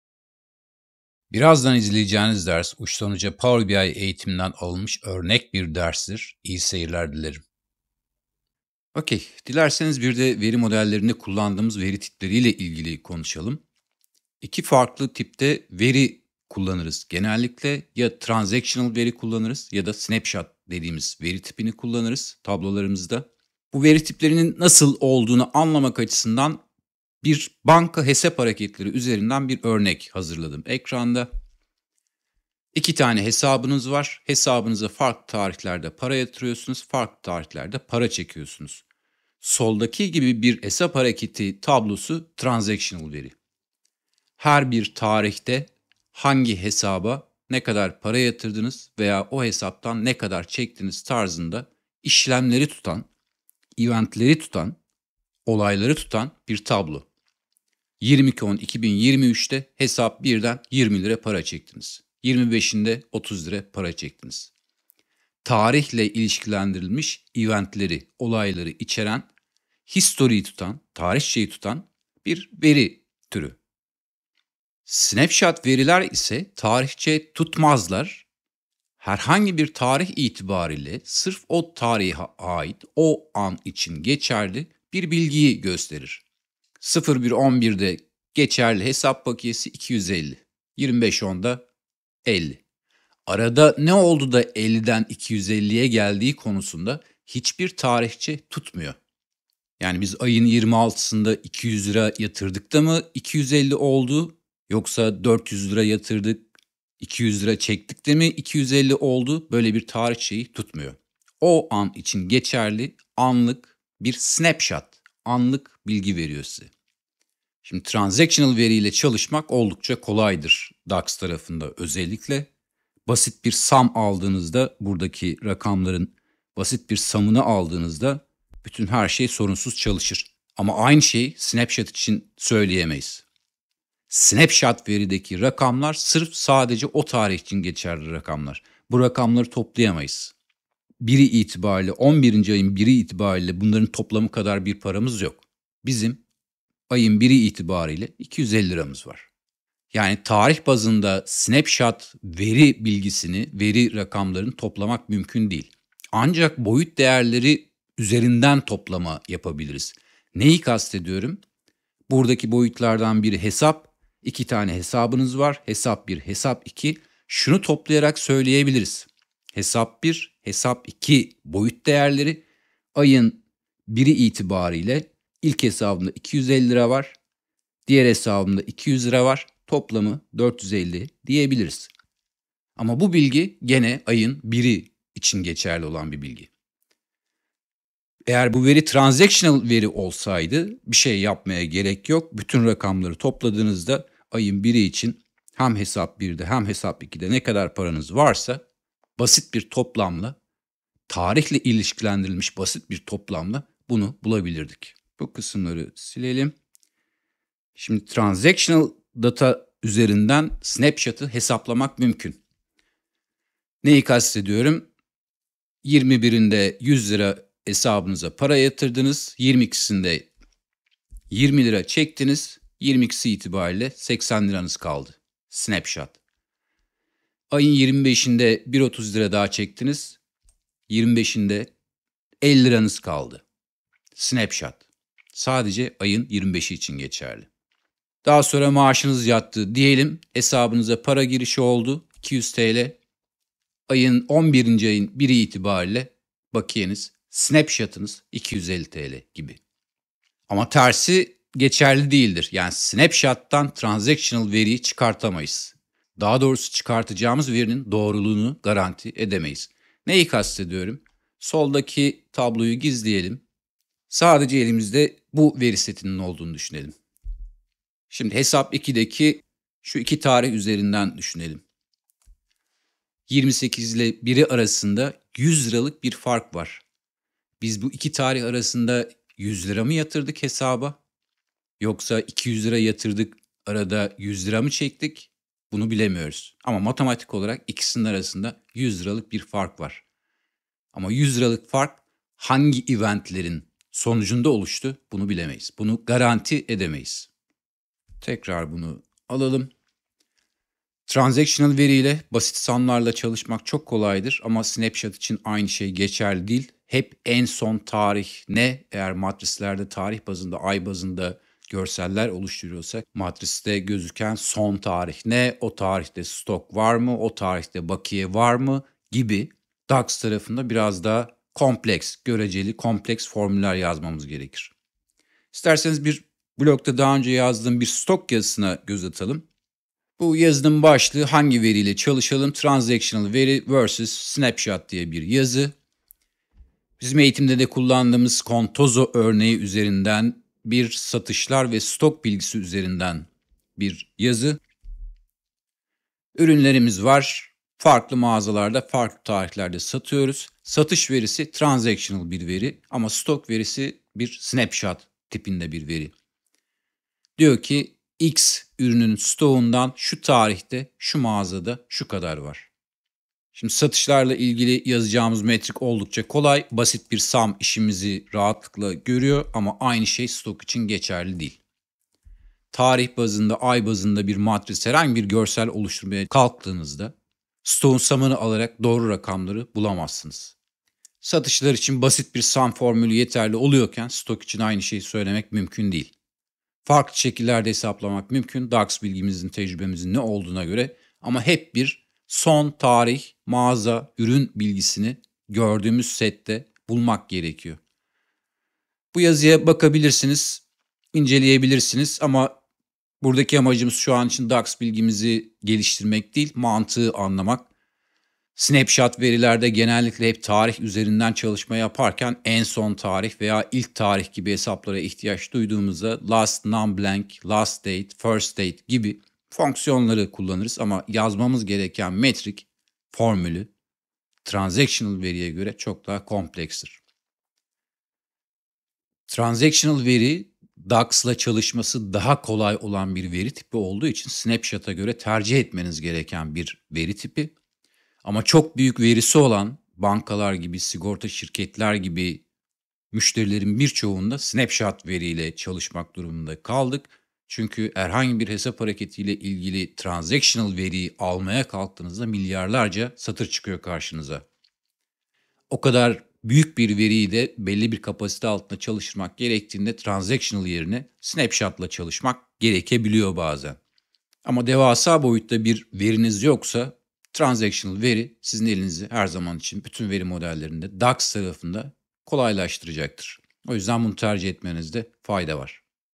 Power BI Eğitimi - Transactional Veri vs Snapshopt
Bu video kaydo Uçtan Uca Power BI eğitimi nden alınmış örnek bir derstir. Eğitim içeriği ve üyelik için ⬇️